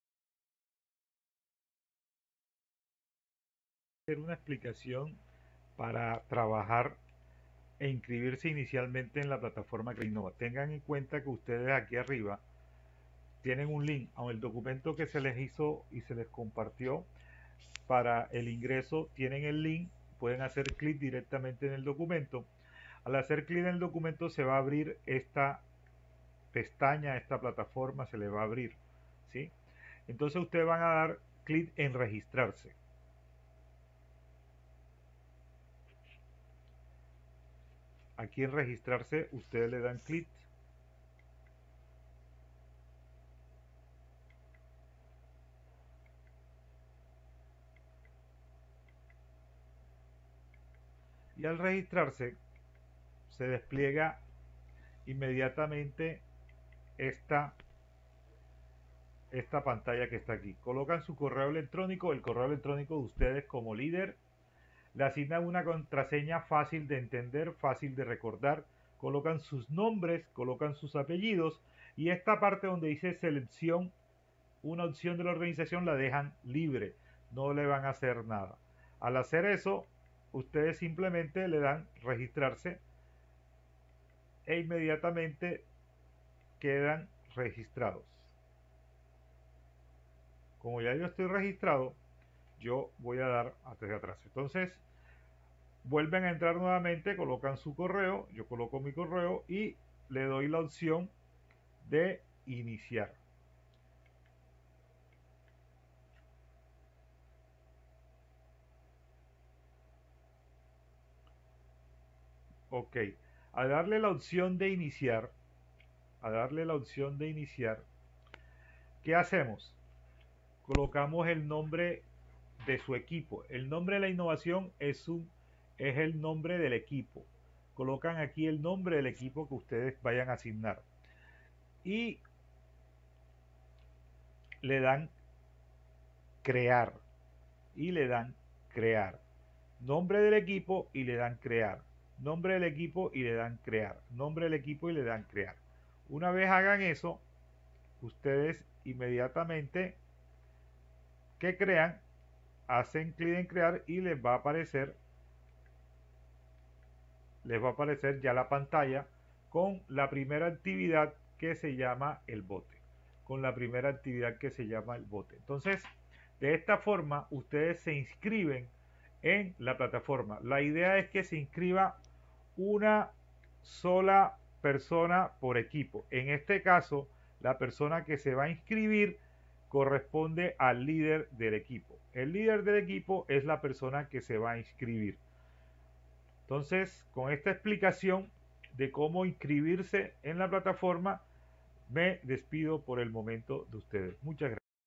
Inscrpcion en INNOCLUSTER
Se muestra la forma para inscribiser en INNOCLUSTER